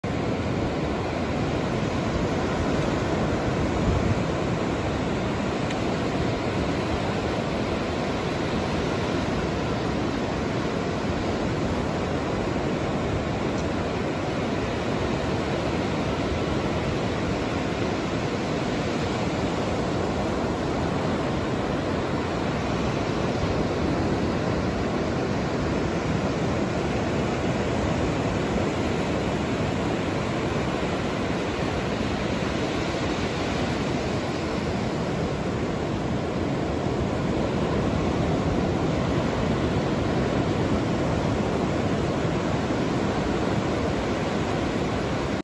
17 June 2024 - Chillin'
Chillin' at Ocean Beach, San Francisco.